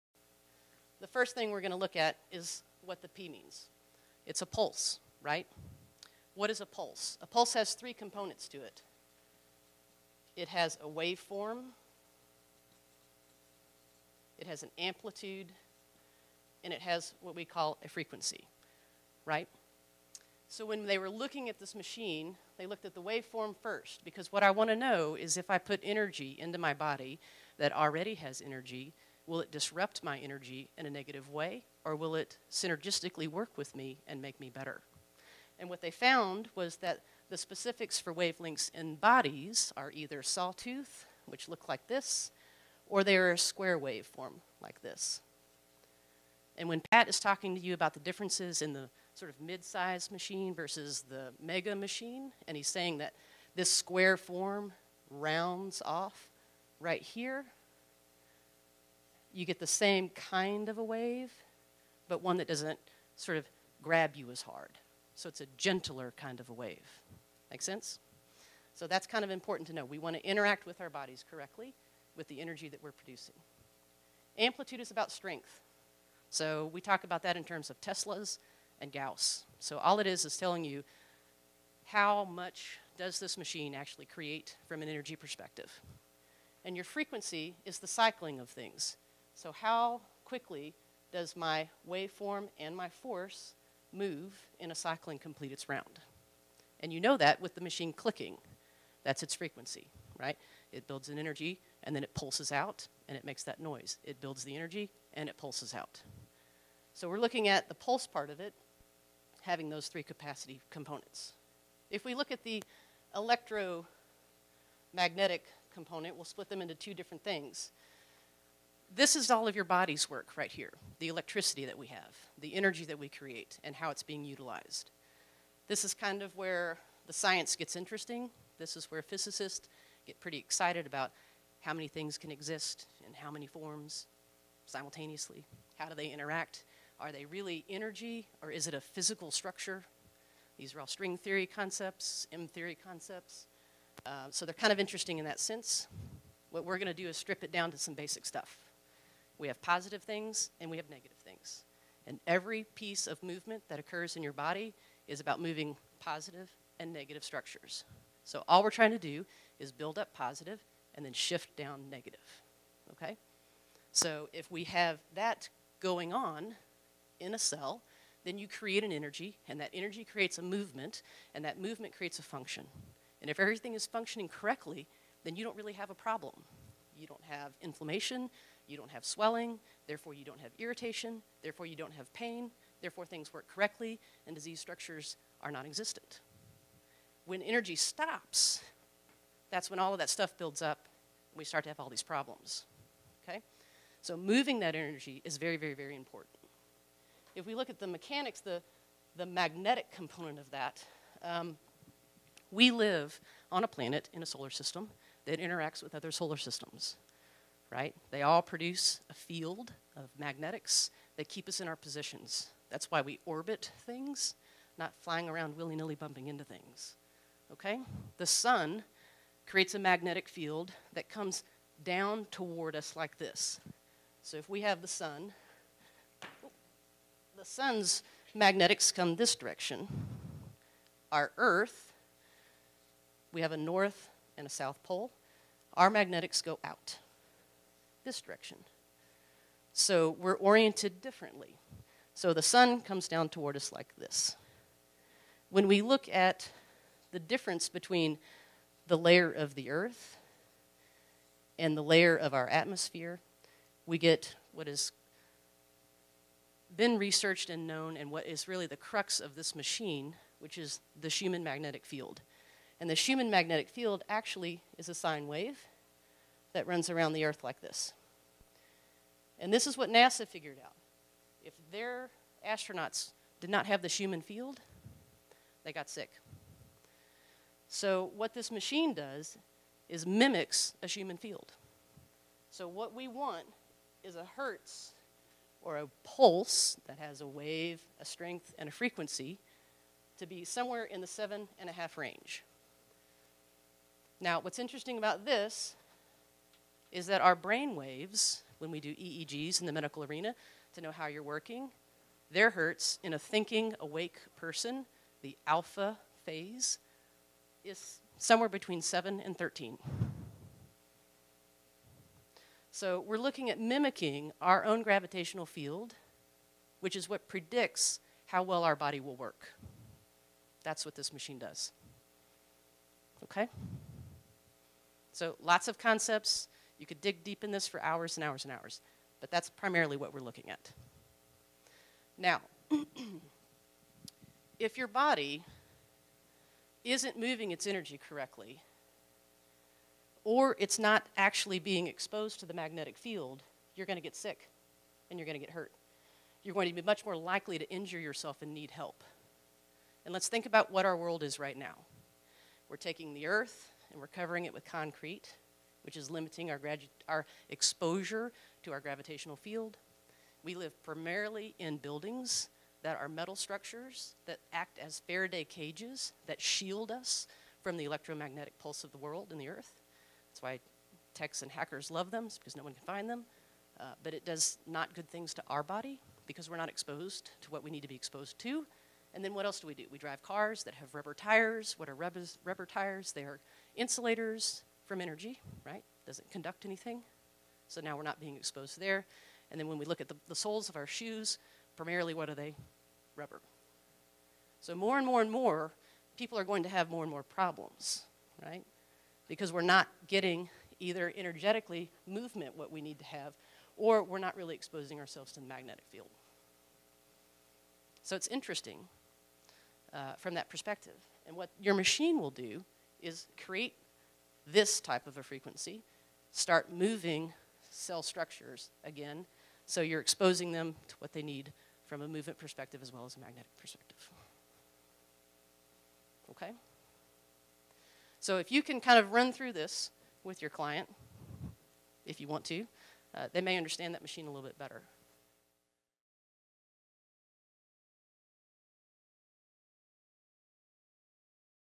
What does PEMF stand for? Dr. Amanda Myers, M.D. explains this term and more about MagnaWaving
Amanda Myers, MD explains what Magna Wave PEMF stands for and why it works. She breaks down the science and explains why PEMF works so well in terms that everyone can understand. Want to know more about how Magna Wave PEMF can benefit you?